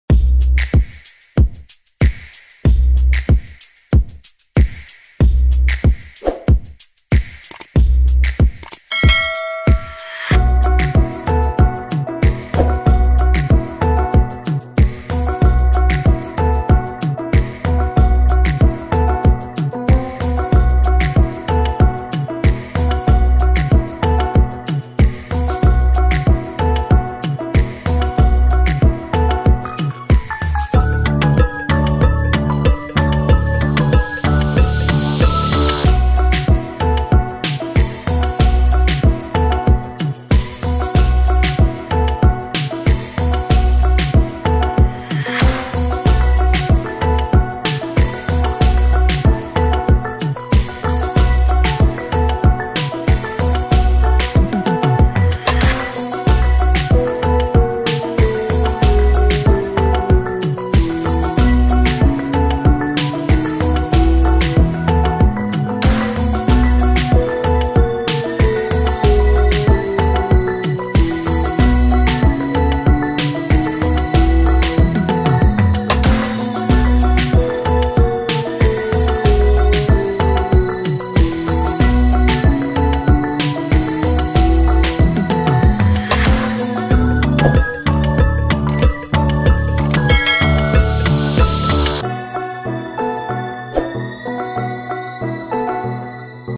วิธีเปิดใช้งานโหมดมืดบน Instagram (หลังจากอัปเดตการตั้งค่าใหม่) 2023
ในวิดีโอนี้ คุณจะได้เรียนรู้ [ วิธีเปิดใช้งานโหมดมืดบน Instagram หลังจากอัปเดตการตั้งค่าใหม่ ]  ดูวิดีโอแบบเต็มนี้และทำตามขั้นตอนทั้งหมดของฉันโดยไม่ข้าม  เป็นวิธีที่ง่ายและสะดวกสุดๆ